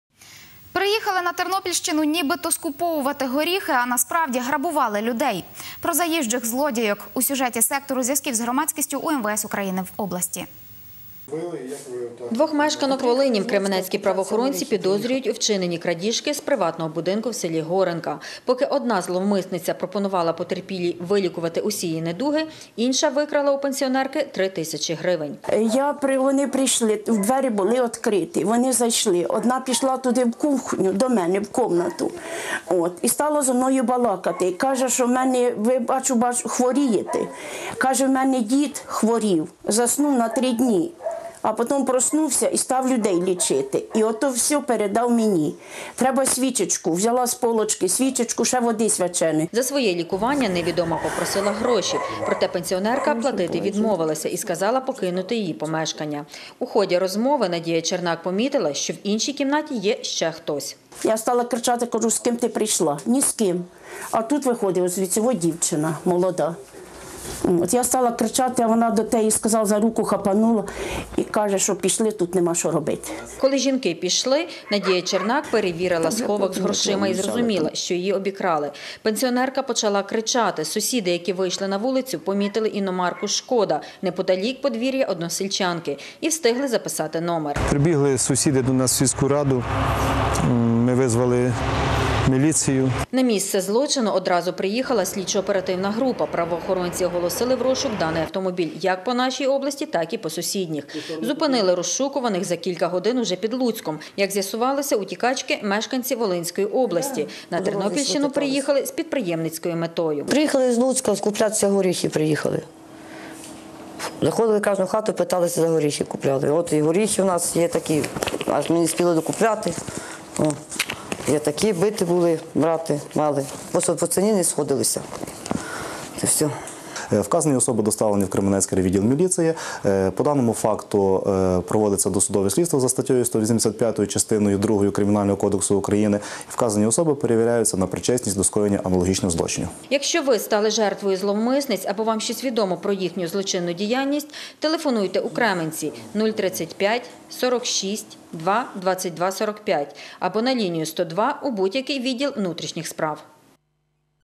На Тернопільщині впіймали заїжджих злодійок
Приїхали на Тернопільщину, ніби-то скуповувати горіхи, а насправді – грабували людей. Про заїжджих злодійок – у сюжеті сектору зв’язків з громадськістю УМВС України в області.